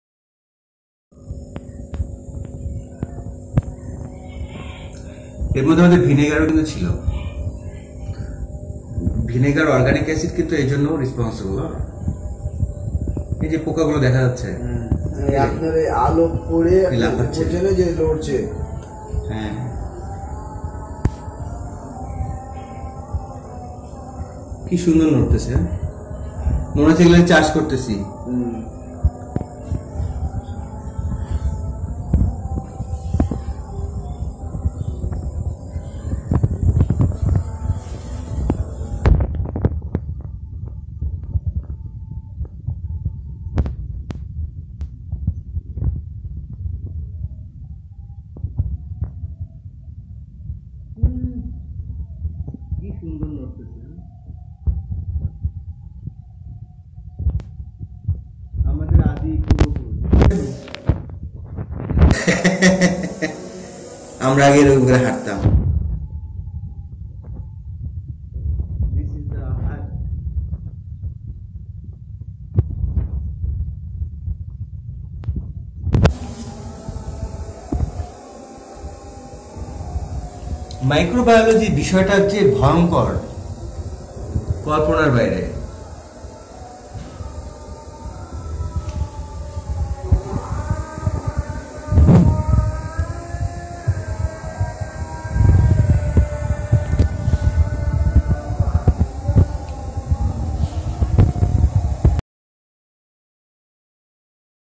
Floc. Biofloc Tank monitoring to avoid conflict (What happen if settled down) On date 2 Dec 2019
It was 2 December 2019, without fish we had produced floc and after one month we had drained all water to observed floc condition. We were surprised, when we closely seen some worm already produced and moving thorough the floc!
This video will helpful to those whose are going to start Biofloc fish farming and under concern about floc..